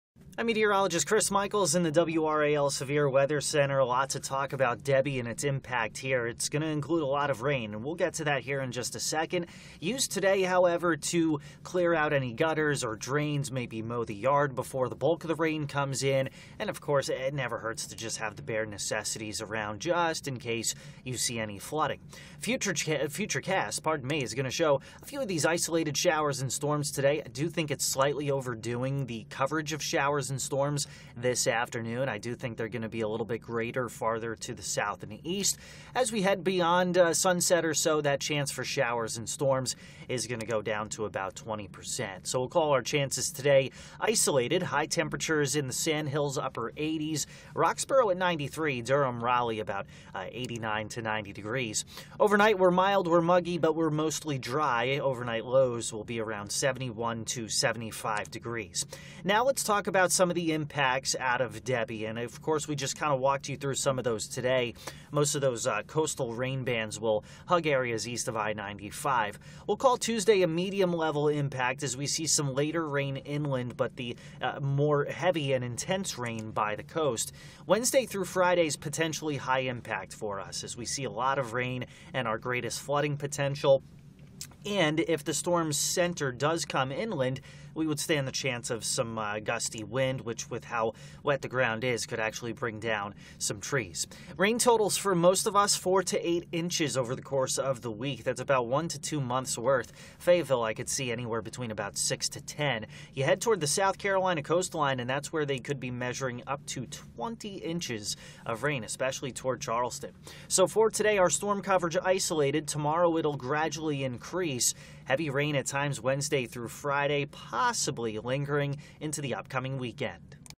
North Carolina Forecast: Calm start Monday before rain from Debby hits NC this week
After some areas of Wake County saw torrential rain Sunday afternoon and early evening, we're in for a calm start to the morning Monday. Rain water is still sitting on some roads, so if you're traveling overnight, take things easy.